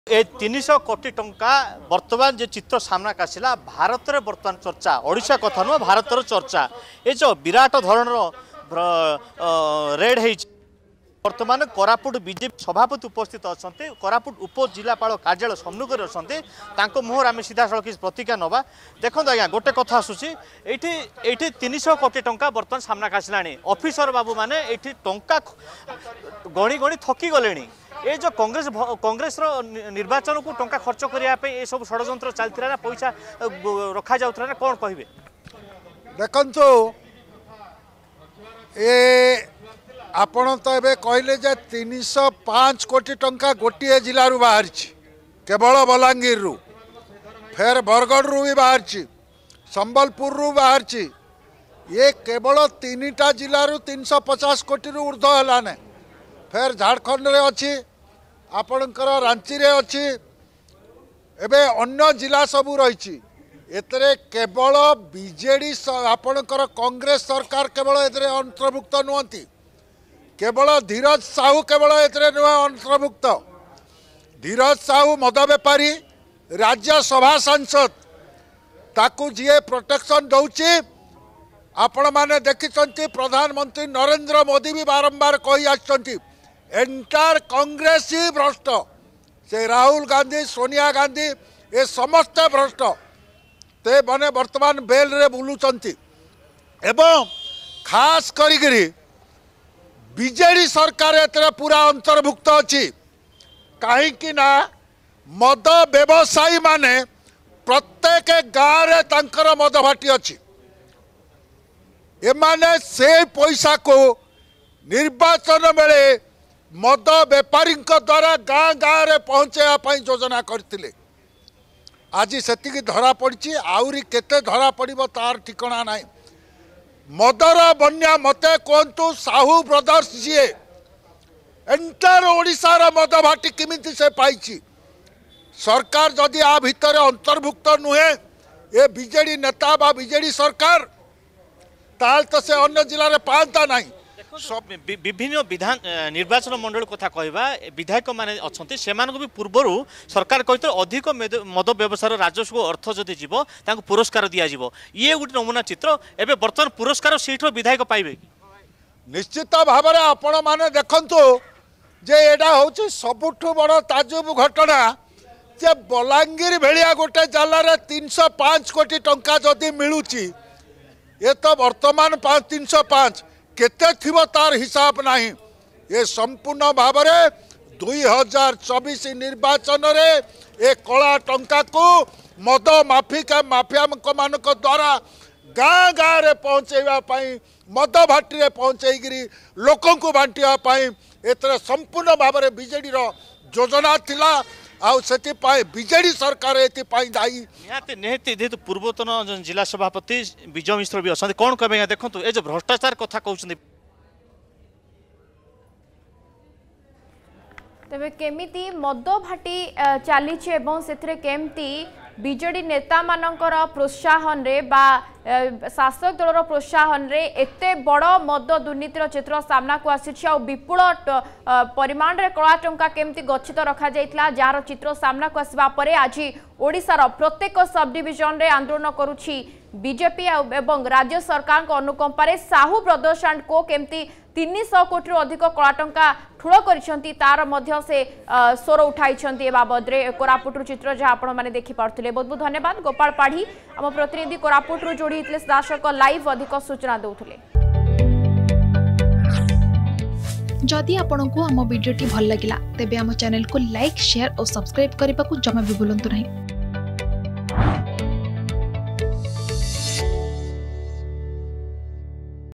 ମଦ ଦୁର୍ନୀତି ଓ କଳା ଟଙ୍କା ବିରୋଧରେ କୋରାପୁଟ ବିଜେପି ପକ୍ଷରୁ ଆନ୍ଦୋଳନ || BJP protest
ମଦ ଦୁର୍ନୀତି ଓ କଳା ଟଙ୍କା ବିରୋଧରେ କୋରାପୁଟ ବିଜେପି ପକ୍ଷରୁ ଆନ୍ଦୋଳନ ।